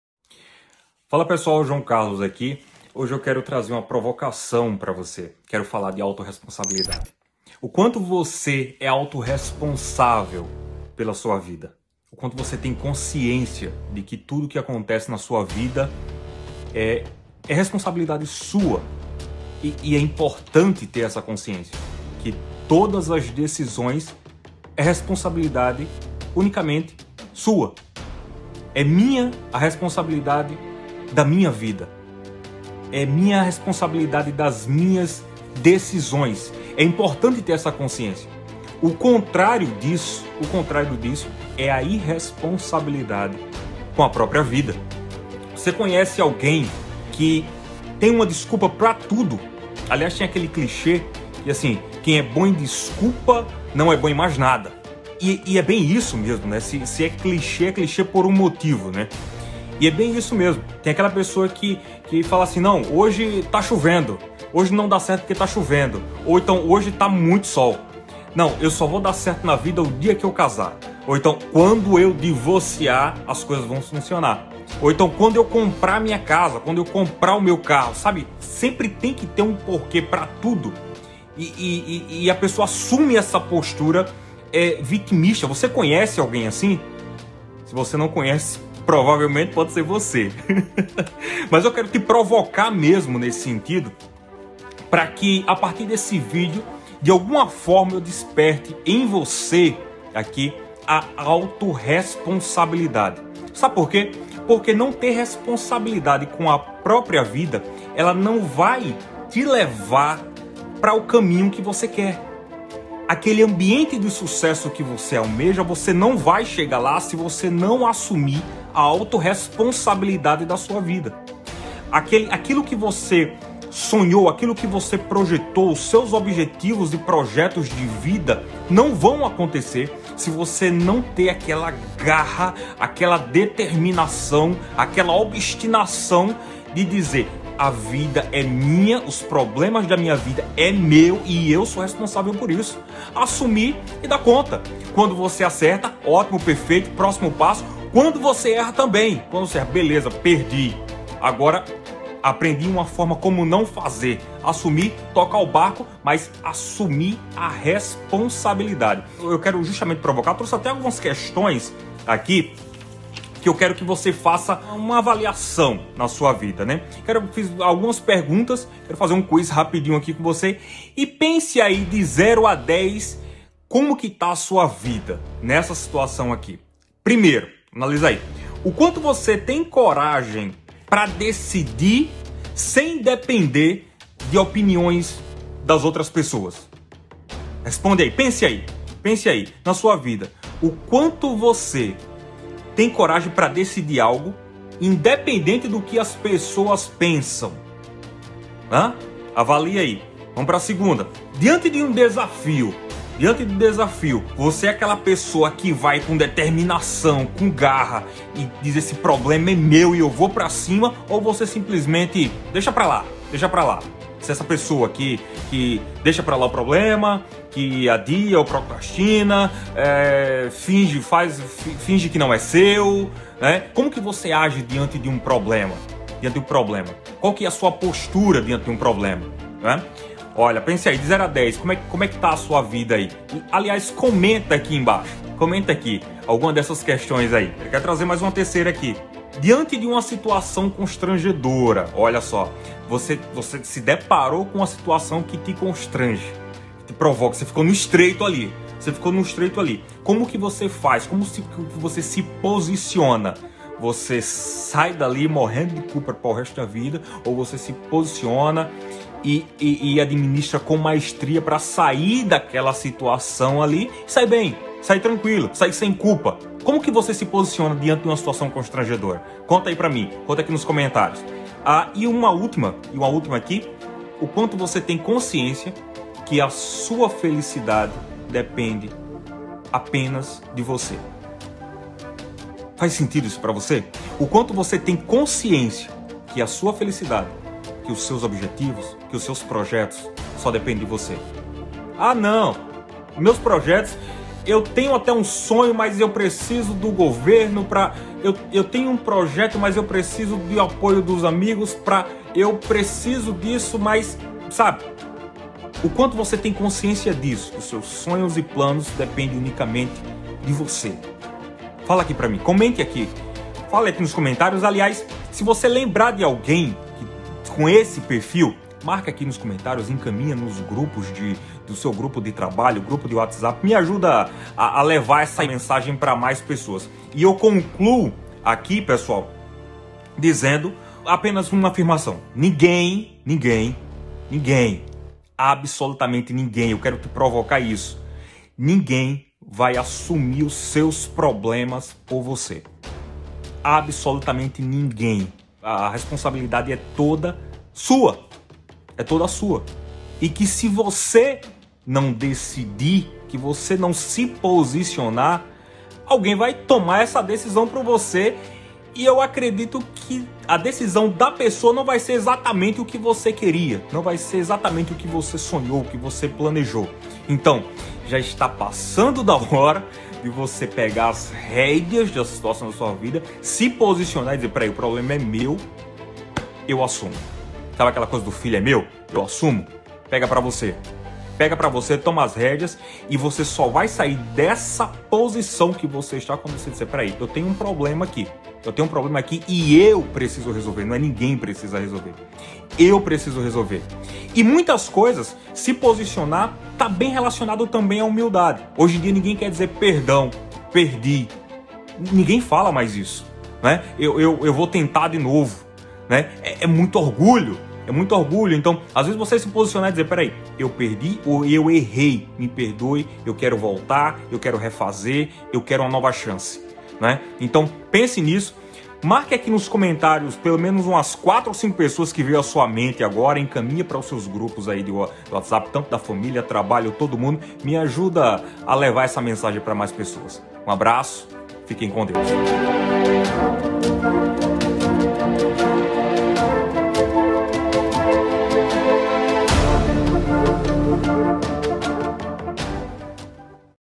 A importância da auto responsabilidade | Negocio dos Grandes #01
Olá! Que bom que voce esta por aqui.

Neste vídeo quero desperta você acerca da importância de auto responsabilidade. Se você deseja algum protagonismo em alguma área de sua vida, você precisa ter consciência de que você precisa se posicionar e assumir as rédeas de sua vida.

Se esse vídeo de alguma forma fez sentido pra você, me ajude a entregar essa mensagem a mais pessoas.

Estou me comprometendo a postar vídeos semanais que irão te impulsionar direto para o seu lugar desejado.